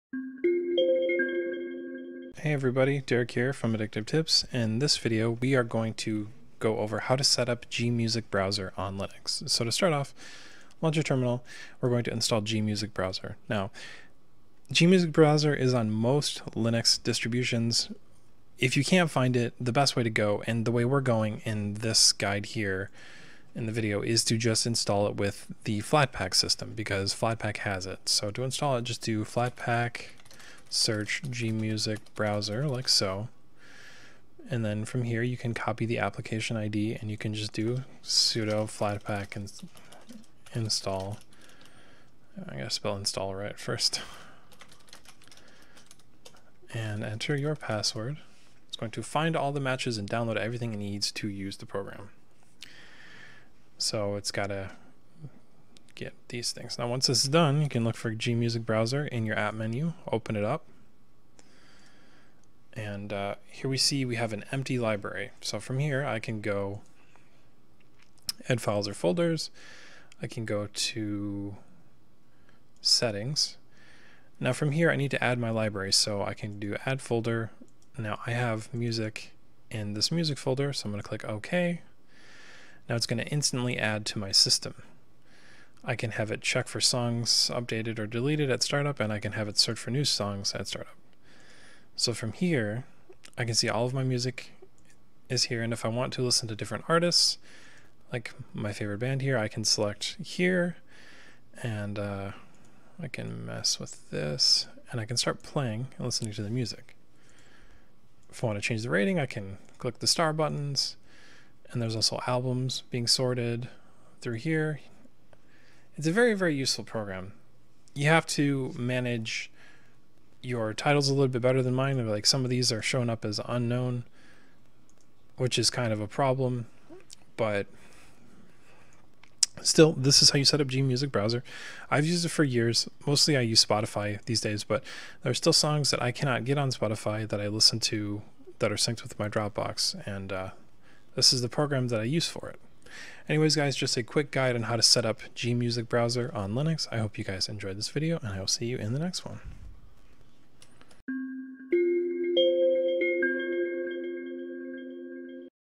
How to set up gmusicbrowser on Linux
Gmusicbrowser is an open-source Jukebox and music collection organizer for Linux. It’s specially designed for large libraries and loads of different songs. In this guide, we’ll show you how to set up Gmusicbrowser on Linux.